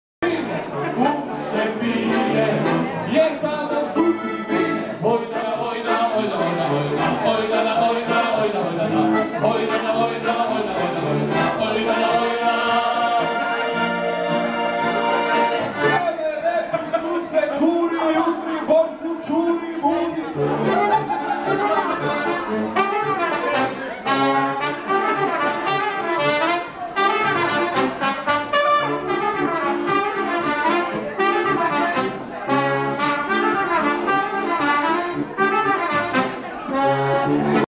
Kongres2015, Družabni del v restavraciji Skriti kot v podhodu Ajdovščina
Glasba: Brkinski trio